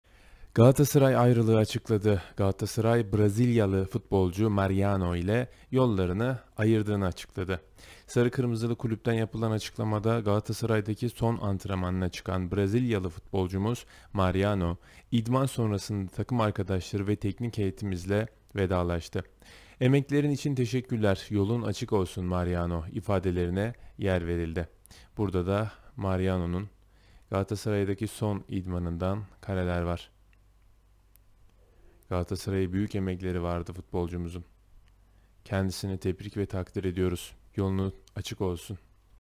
Galatasaray ayrılığı açıkladı
22 Temmuz 2020 Çarşamba 13:13 - Son Güncelleme 22 Temmuz 2020 Çarşamba 13:19
Galatasaray, Brezilyalı futbolcu Mariano ile yolların ayrıldığını açıkladı.

facebookta paylaş

Sarı kırmızılı kulüpten yapılan açıklamada ''Galatasaray'daki son antrenmanına çıkan Brezilyalı futbolcumuz Mariano, idman sonrasında takım arkadaşları ve teknik heyetimizle vedalaştı. Emeklerin için teşekkürler. Yolun açık olsun Mariano.'' ifadelerine yer verildi.